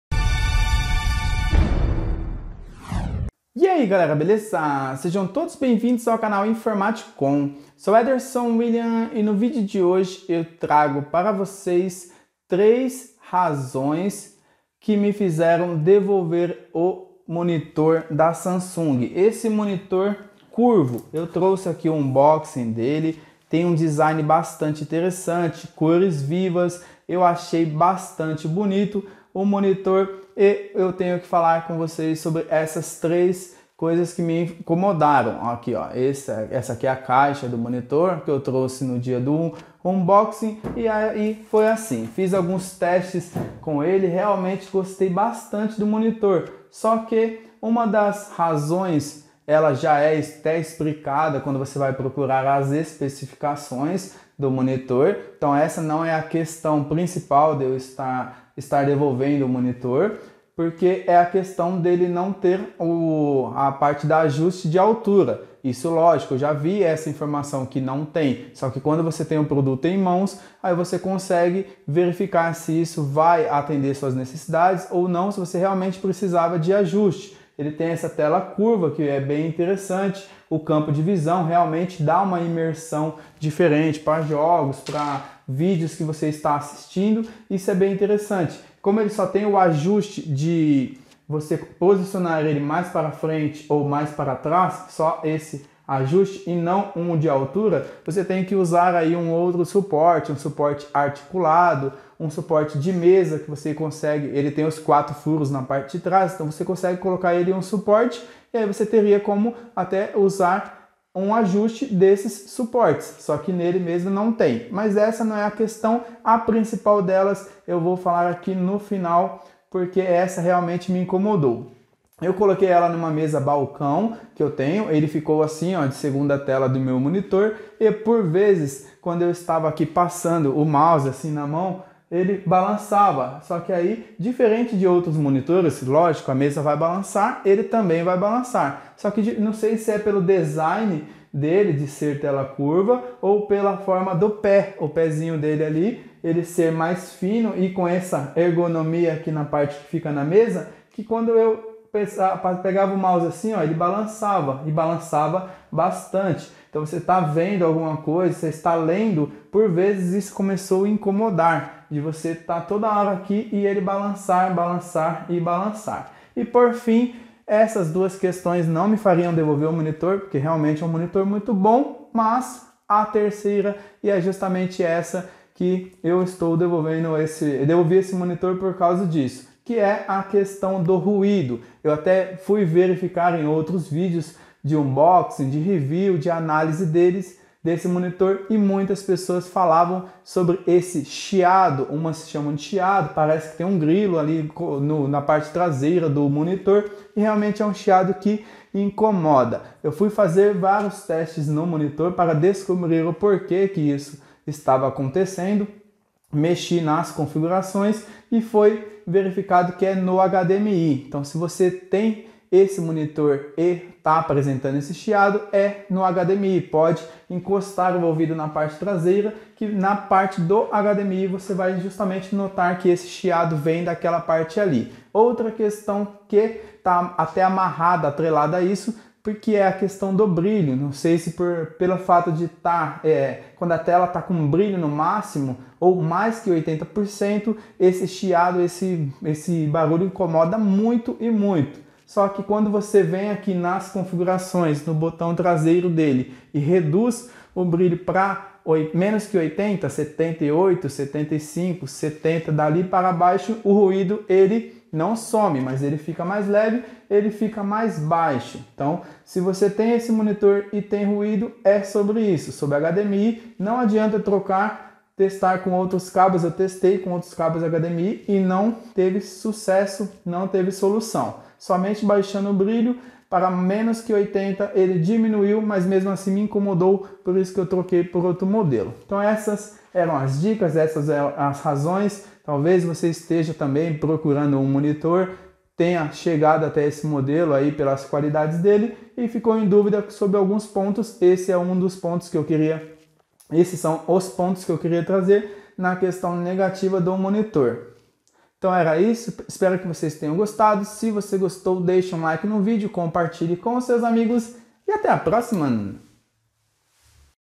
DEVOLVI O MONITOR FULL HD SAMSUNG LED CURVO 24 POLEGADAS MODELO C24F390F EM MENOS DE 24 HORAS
Veja nesse vídeo 3 motivos que me fizeram devolver o monitor Full HD Samsung LED Curvo 24 Polegadas Modelo C24F390F em menos de 24 horas. Apesar do monitor trazer uma excelente qualidade de imagem e imersão em jogos com o design da tela curva, o monitor não tem ajuste de altura, somente pequenos ajustes de inclinação, outra questão a mencionar é sobre os balanços constantes mesmo com pouco movimento da mesa e por fim o defeito de chiado/ruído na saída traseira do monitor, em uma área próxima a conexão HDMI.